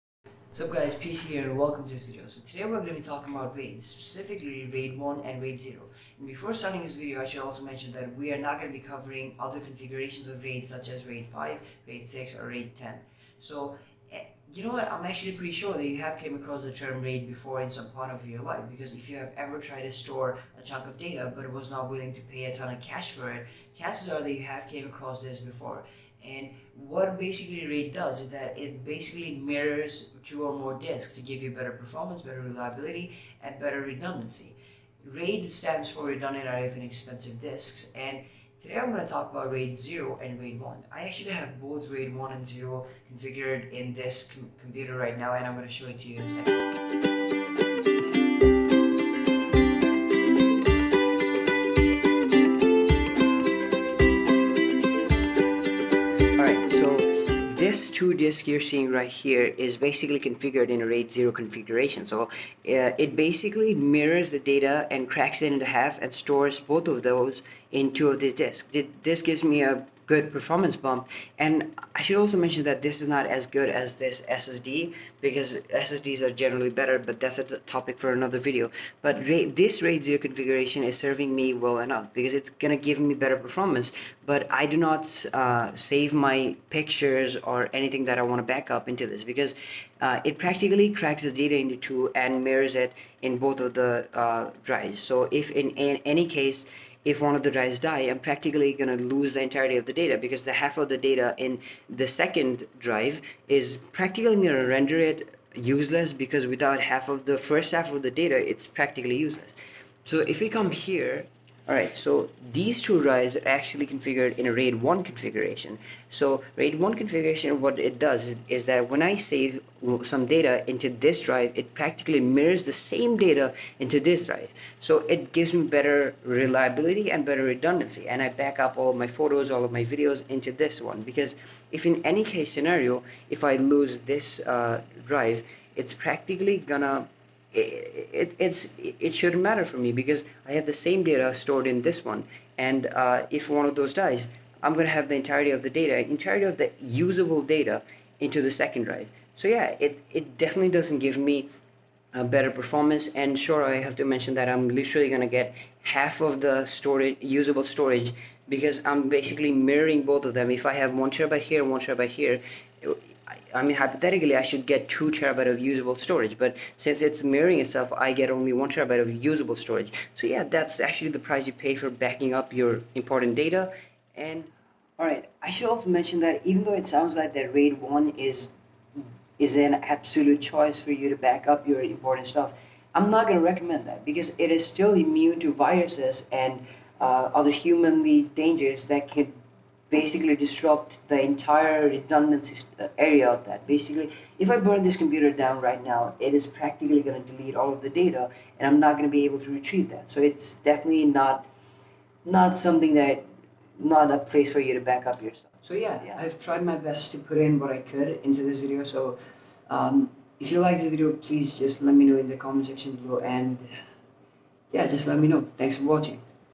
RAID 0, RAID 1 explained in 3 minutes
RAID is a very popular storage option for a lot of people and has been getting a lot of attention in the last couple of decades. I tried to explain RAID 1 and RAID 0 in this video. I hope you like it.